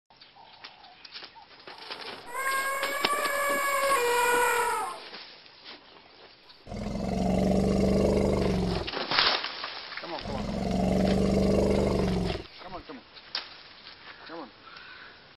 Brave man stops a charging Elephant
This incredible footage shows a brave man stopping an angry charging Elephant using only his stick!